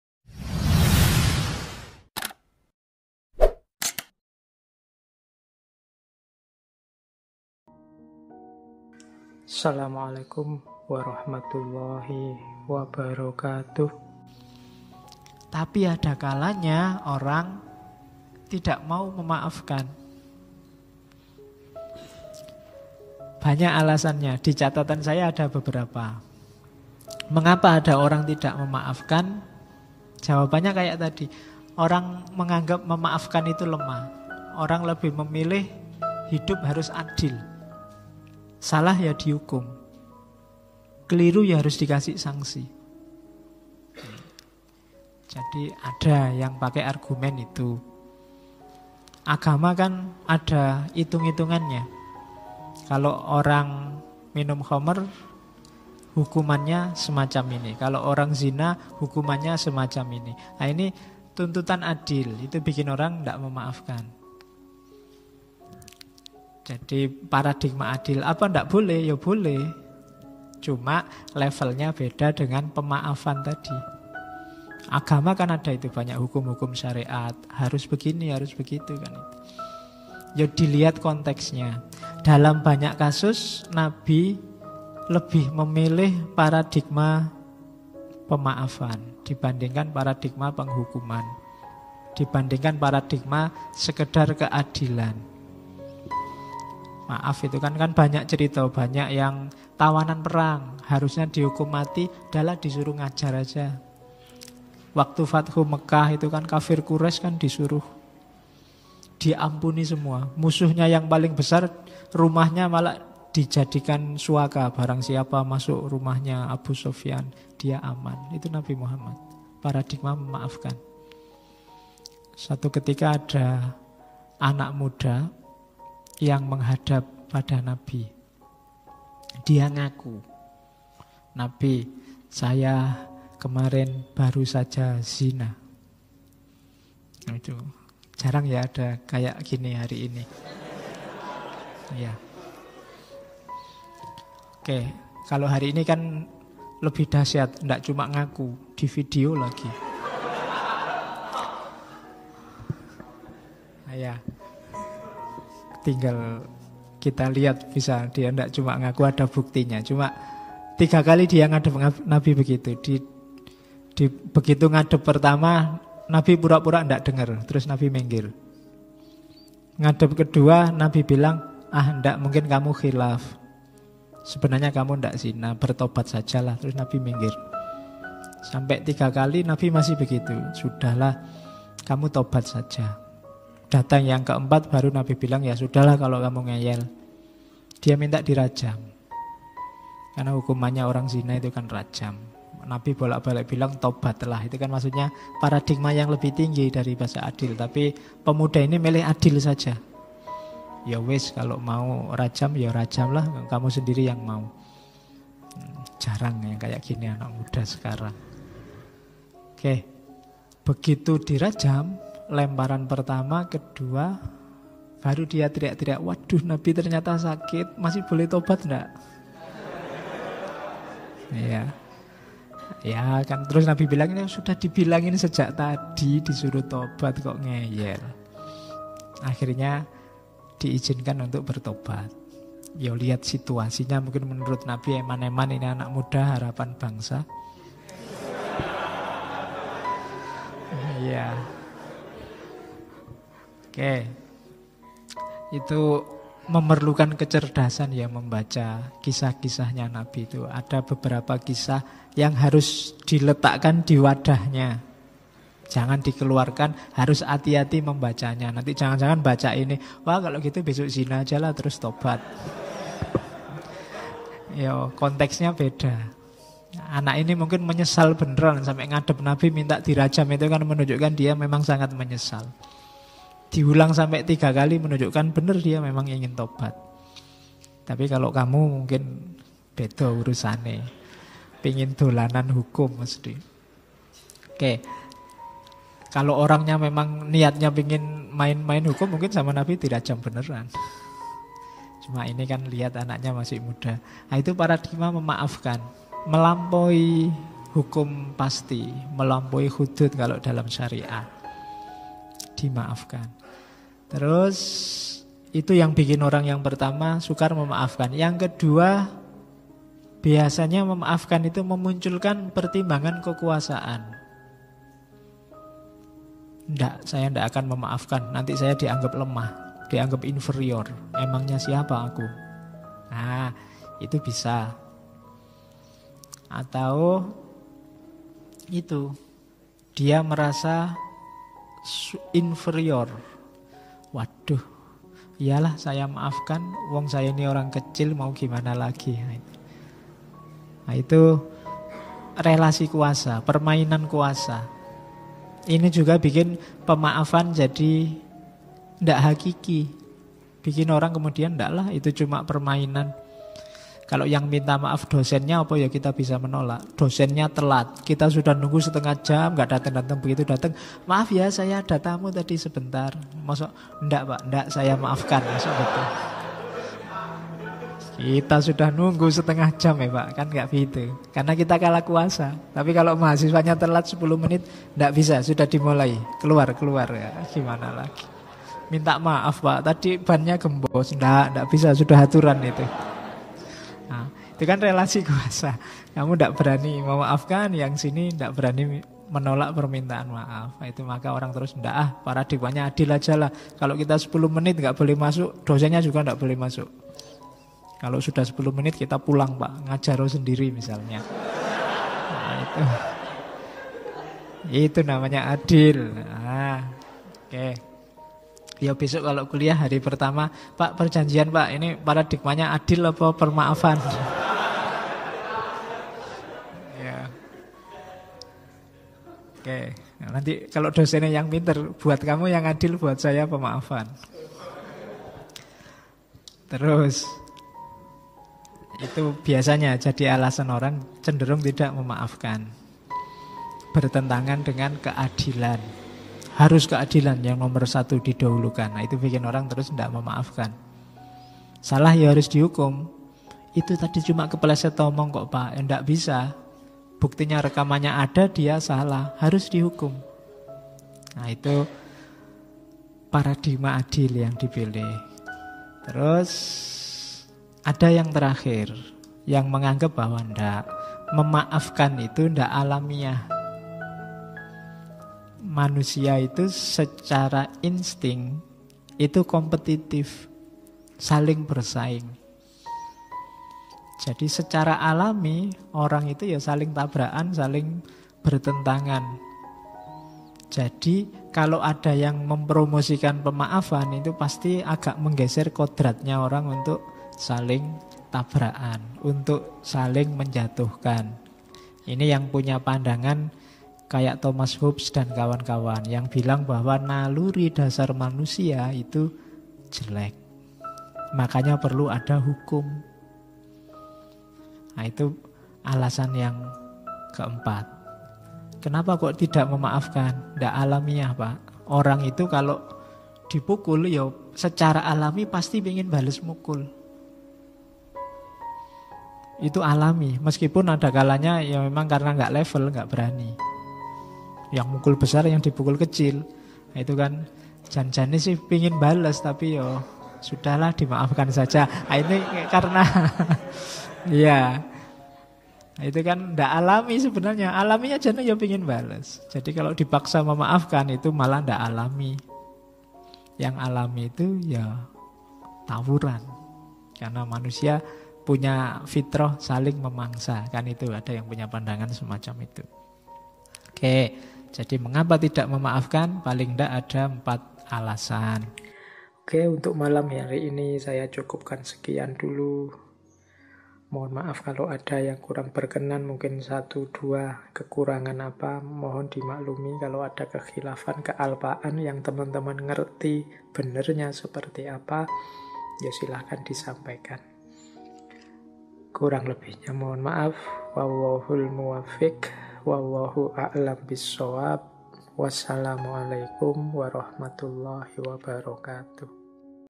4 Alasan Kamu Tidak Mau Memaafkan | Ngaji Filsafat | Dr. Fahruddin Faiz
Dianjurkan menonton sampai selesai, agar tidak terjadi kesalah pahaman. Terima kasih